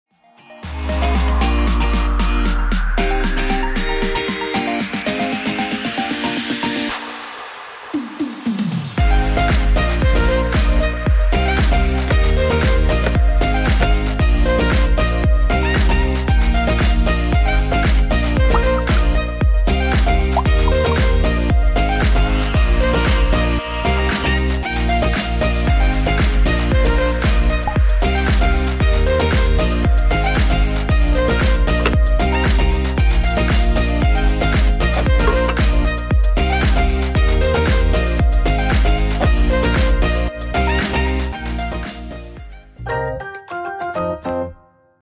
Gaspacho frais et rapide
La préparation du gaspacho classique n'est pas réservée aux Espagnols. Grâce à ALDI, vous pouvez vous aussi préparer en un rien de temps ce potage froid andalou à la tomate avec une touche épicée. Regardez notre vidéo astucieuse et goûtez aux vacances !